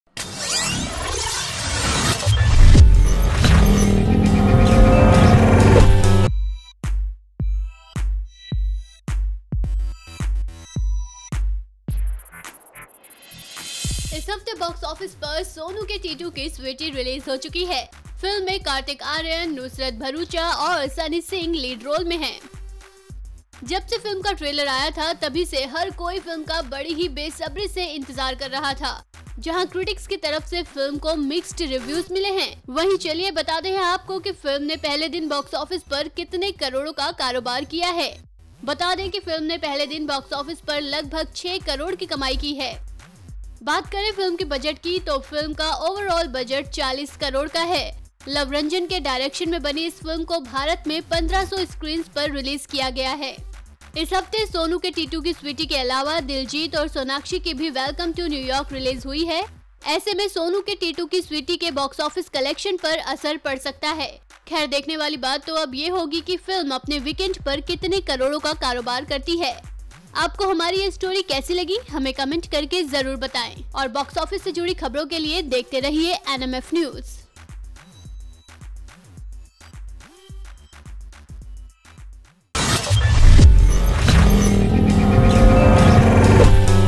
Sonu Ke Titu Ki Sweety ने पहले दिन की मोटी कमाई, आंकड़े जानकर दंग रह जाएंगे आप
Sonu Ke Titu Ki Sweety box office collection day 1: Kartik Aaryan film to earn Rs 6 crore Sonu Ke Titu Ki Sweety box office collection day 1: The film, that releases today, is being marketed as almost a successor to Pyar Ka Punchnama, 2011's cult hit. The director is the same, Luv Ranjan. Kartik Aaryan and Nushrat Bharucha also return.